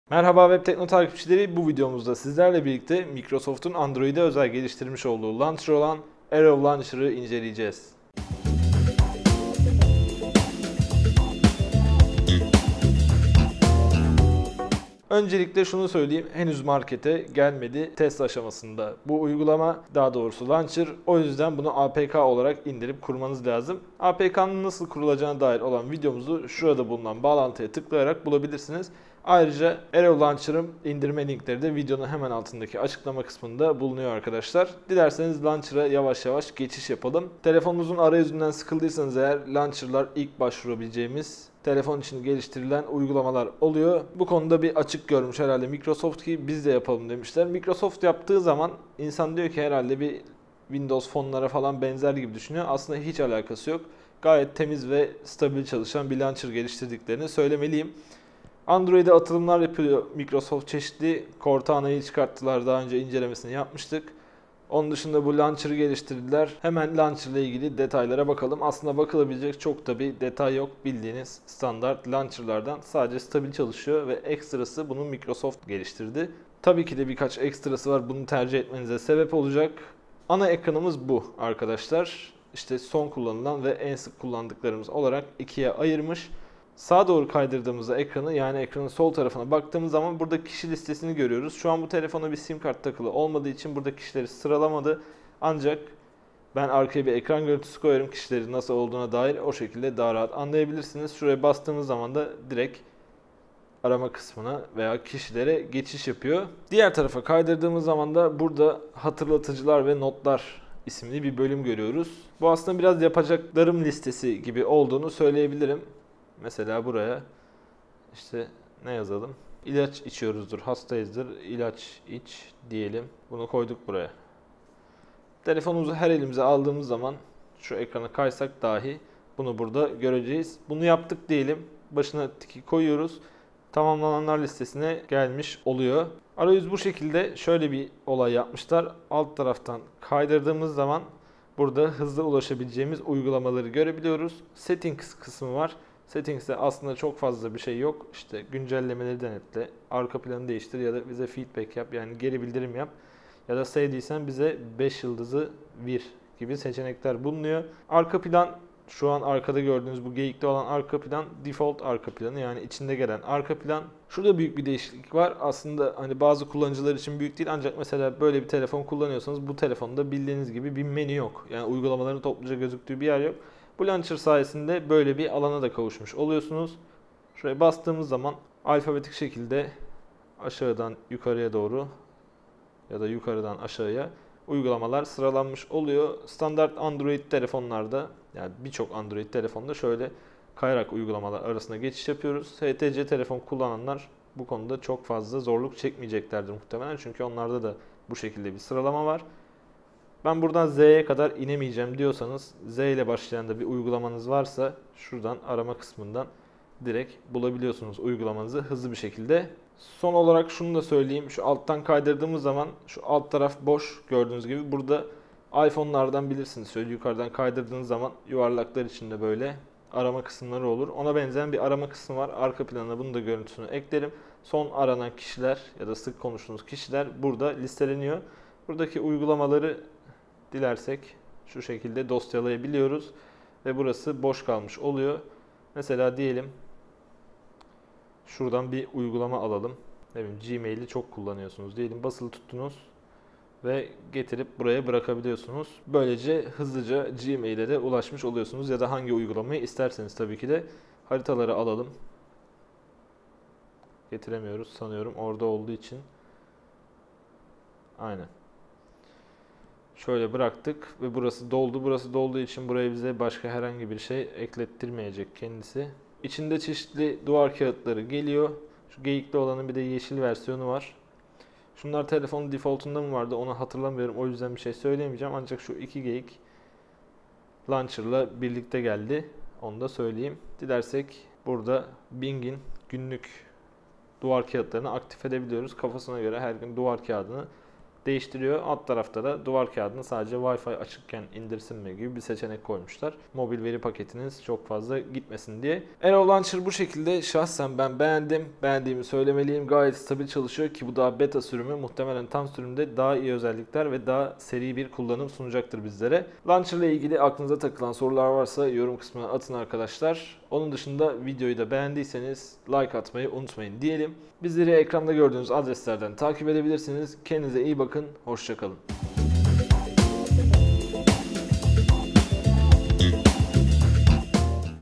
Microsoft Arrow ile Android Arayüzünüzü Değiştirin
Microsoft tarafından Android platformu için üretilen Arrow Launcher'ı inceliyoruz. Beklenildiği gibi Windows Phone arayüzü yerine farklı bir arayüze sahip olan Arrow Launcher'ın detaylarını videomuzda bulabilirsiniz.